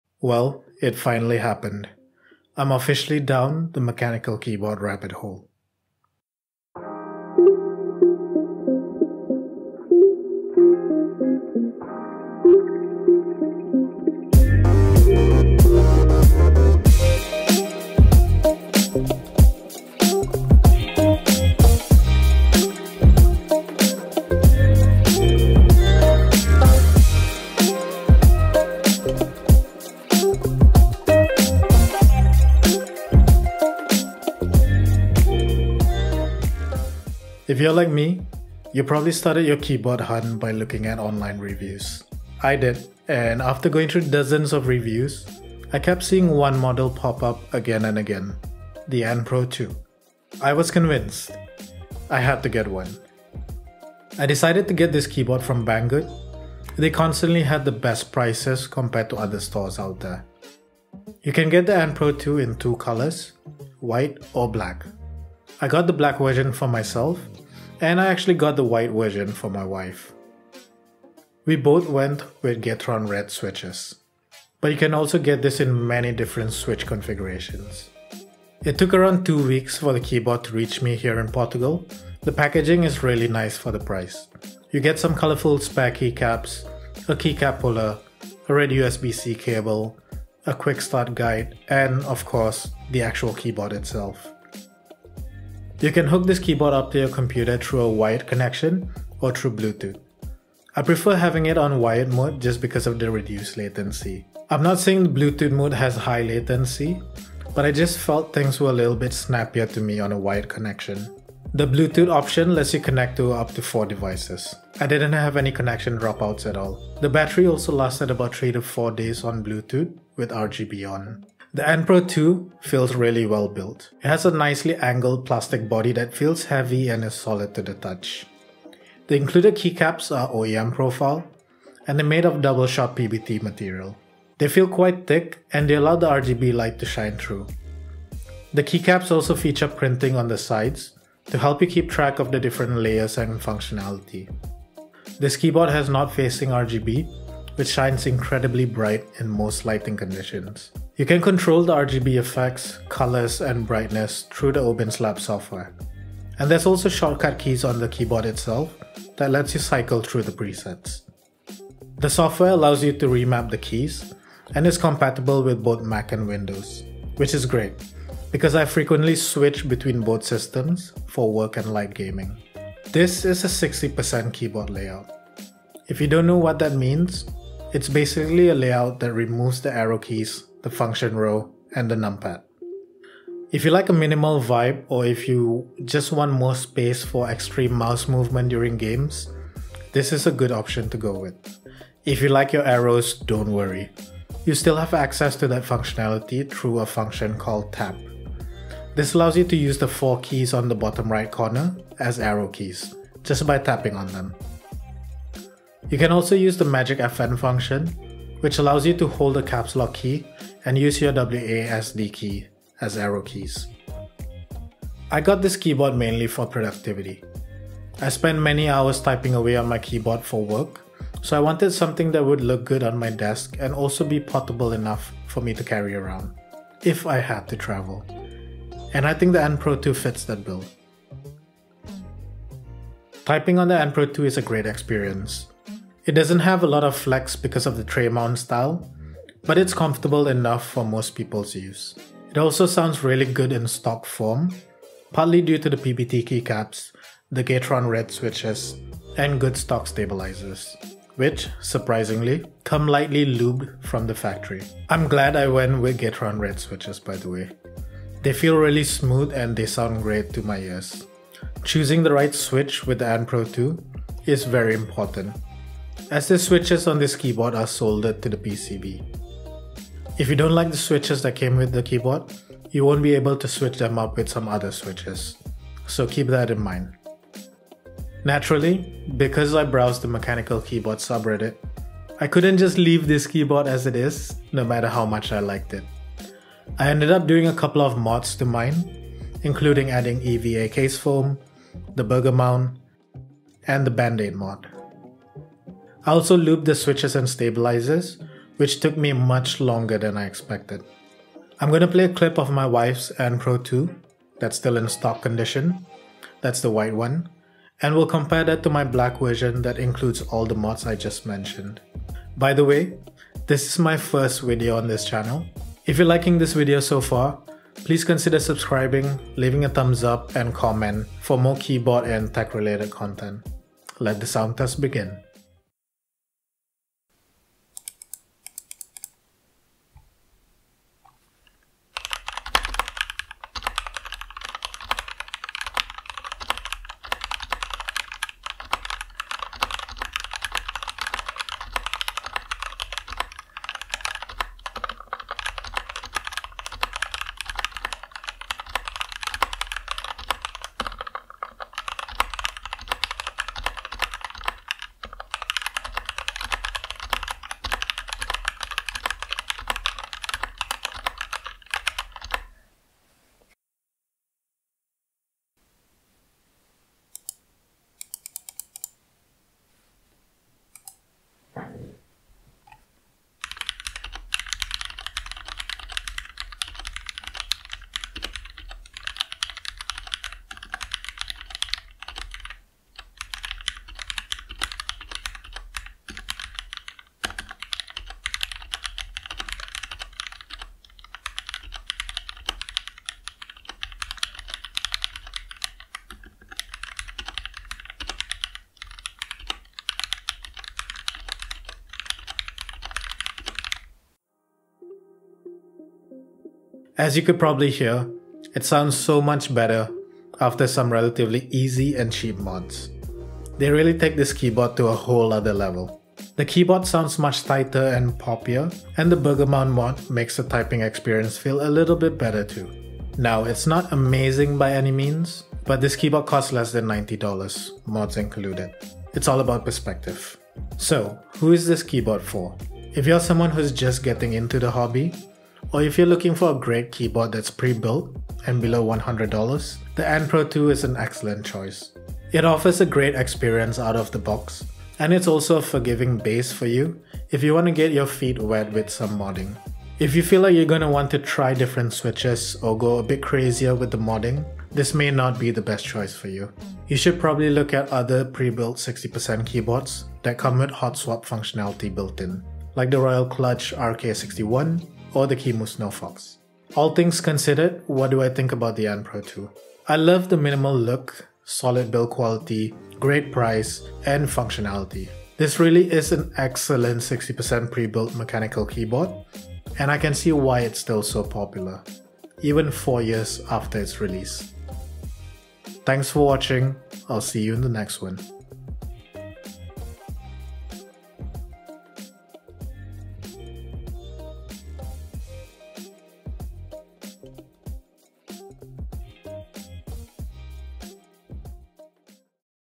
Anne Pro 2 Review | The Best Prebuilt Mechanical Keyboard Under $100?
The Anne Pro 2 is no stranger to the mechanical keyboard scene. It's widely considered to be one of the best prebuilt 60 percent keyboards for Mac and Windows, and the low latency on wired connection makes it an excellent keyboard for gaming too.

How does it fare in 2022, 4 years after it was first released? With the 60% mechanical keyboard market gaining huge ground over the past few years, is the Anne Pro 2 still the best wireless mechanical keyboard under $100?

We'll have a closer look at this affordable keyboard, and we'll go through some sound tests reviewing the Anne Pro 2 in stock vs modded form.

BUY THE ANNE PRO 2:
Anne Pro 2 (Banggood)
Anne Pro 2 (AliExpress)

Some alternative prebuilt 60% keyboards with hot-swap:
Royal Kludge RK61
Akko 3061S Tokyo R2

KEYCAPS:
Dark Olivia Clones (PBT)

Timestamp:

00:00 Intro & B-Roll
00:38 Keyboard hunting
00:54 Purchasing on Banggood
01:18 Packaging
01:36 Connectivity
02:02 Build Quality & Keycaps
02:30 Anne Pro 2 Software (Obinslab)
02:52 What is a 60% keyboard?
03:11 TAP & MAGIC FN for arrows
03:52 Typing Feel & Experience
04:15 Gateron Red Switches
04:47 Mods (EVA Case Foam, Bandaid, Burger Mount)
05:25 Sound Test (Stock Anne Pro 2 - White)
06:06 Sound Test (Modded Anne Pro 2 - Black)
06:46 Thoughts & Conclusion